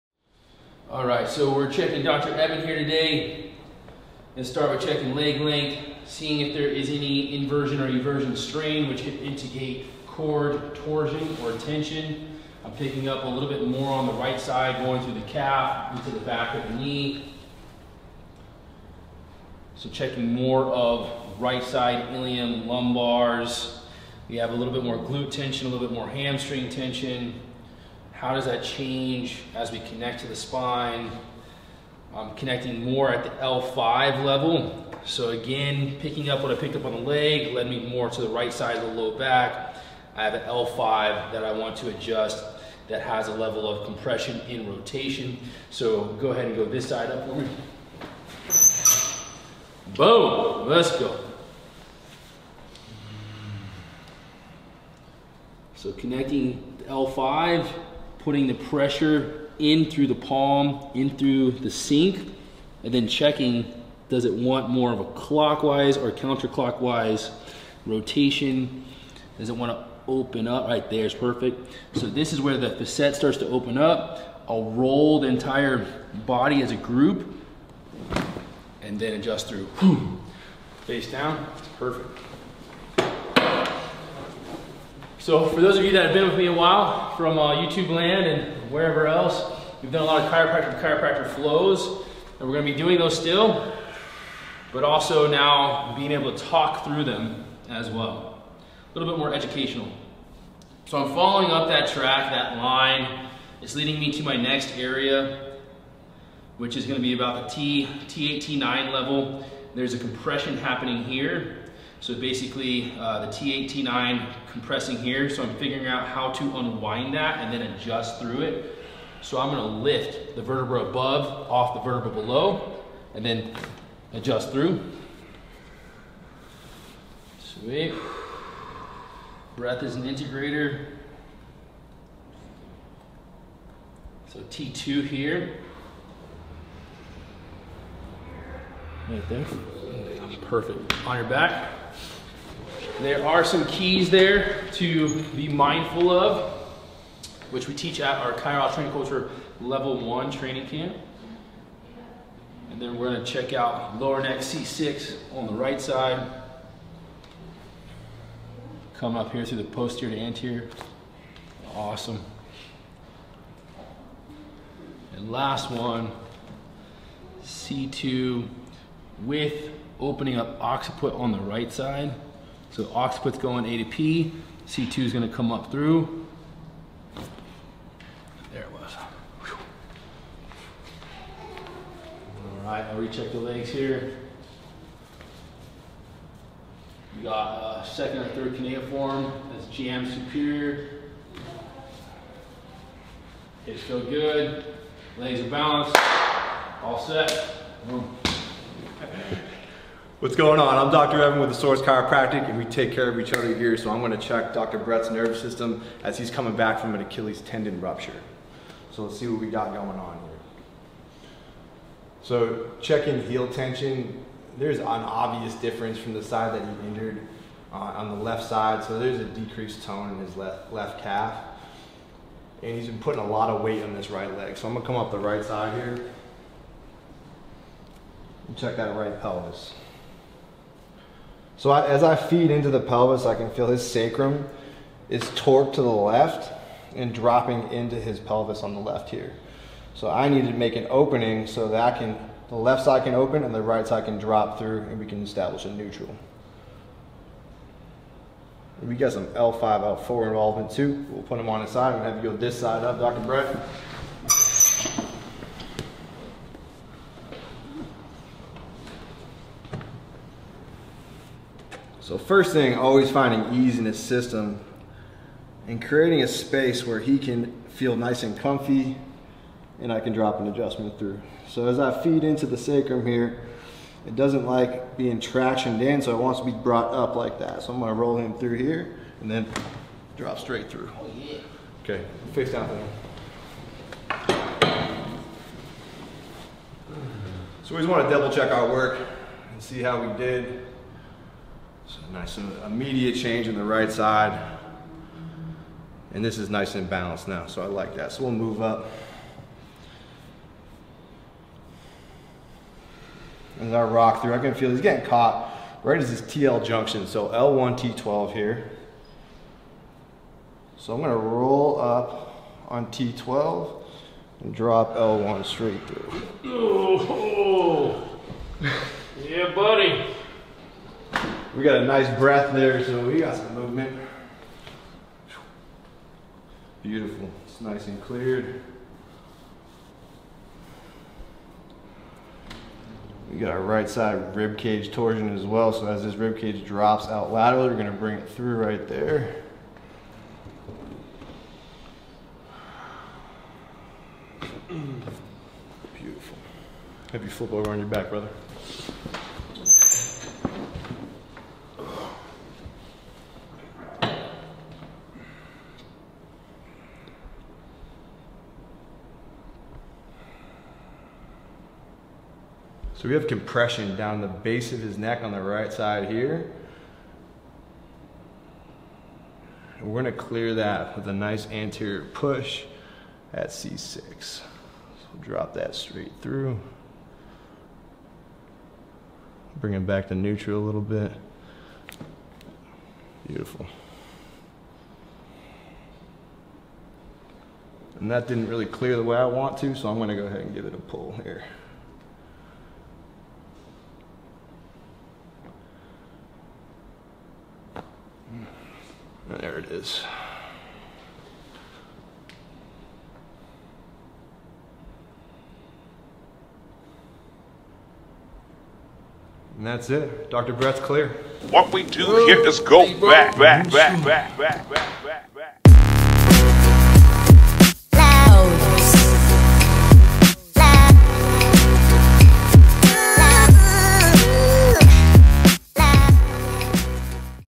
Chiropractor to Chiropractor: Dr. Evin Pingalore and Dr. Brett Jones
Welcome to another edition of Chiropractor to Chiropractor!

Both Dr. Evin Pingalore and Dr. Brett Jones practice together at The Source Chiropractic in Tucson, AZ.

You can find out more about their offerings by heading